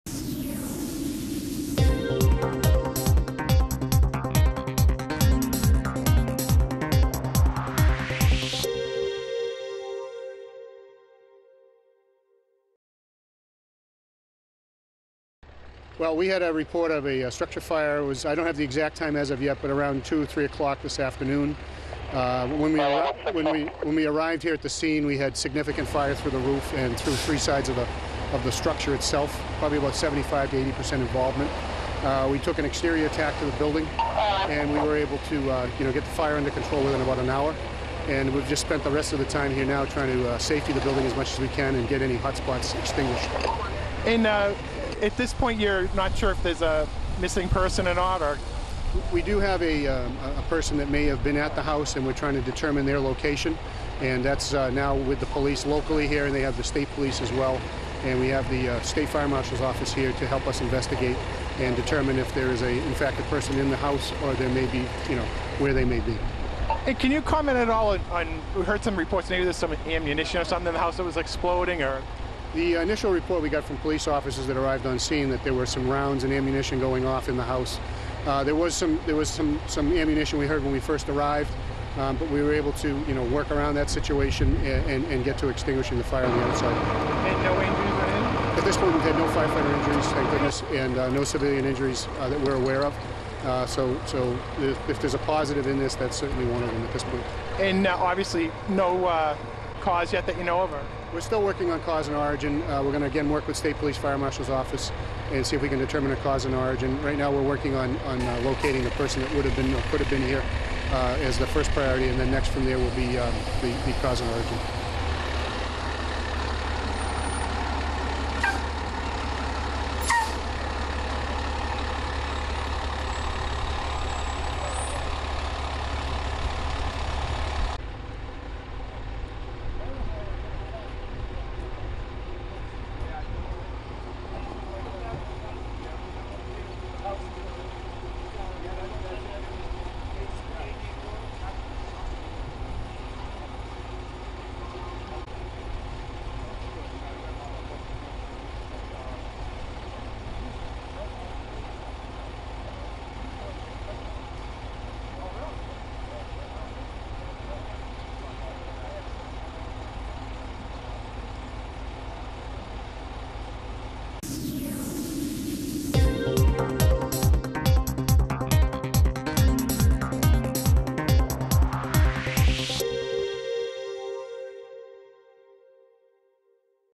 Fatal Fire- Eastham, MA (03-18-12)
According to the State Fire Marshall's Office, one person was killed in a house fire on Gimlet Road in Eastham, Massachusetts Sunday (03-18-12) afternoon.  (Photo by David G. Curran)